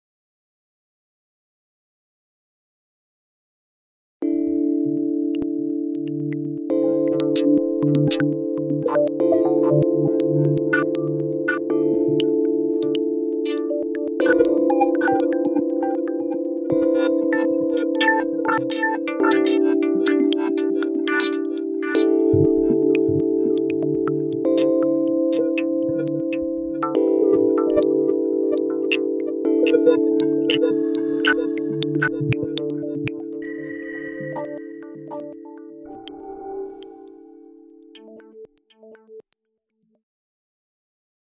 Binary Deutschland
Bored, so I converted "Deutschland" into binary and then binary into the C Major scale.  Blips and bubbles in the background are for effect.